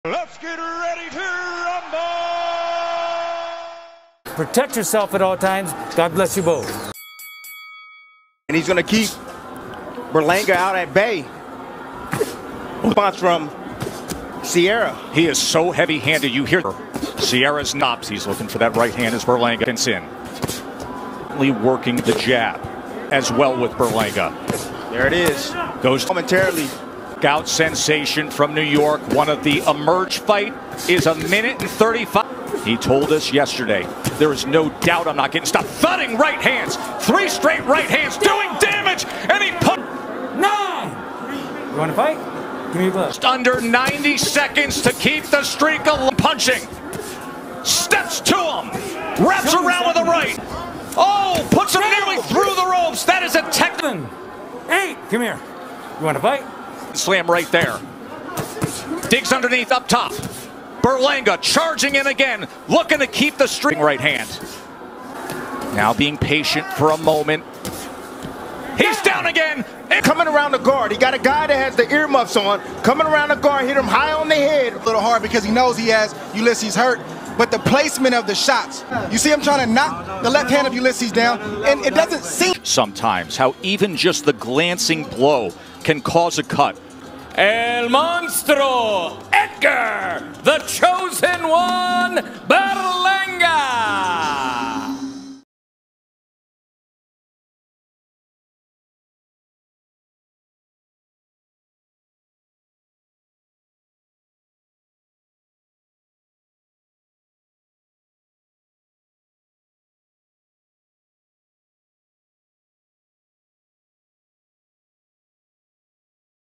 Edgar Berlanga Vs Ulises Sierra | Full Fight Highlights | BOXING FIGHT | HD
Experience the electrifying clash between Edgar Berlanga and Ulises Sierra in this must-watch full fight highlight reel! Berlanga, known for his incredible knockout streak, faces off against the resilient Sierra in a battle that showcases both power and skill. Watch as Berlanga brings his trademark ferocity to the ring, pushing Sierra to his limits with relentless pressure and explosive punches. This fight is a true test of endurance and strategy, with both fighters delivering unforgettable performances. Whether you're a fan of Berlanga's knockout power or Sierra's determination, this highlight is packed with action from start to finish. Don't miss the chance to relive every punch, dodge, and knockout in HD!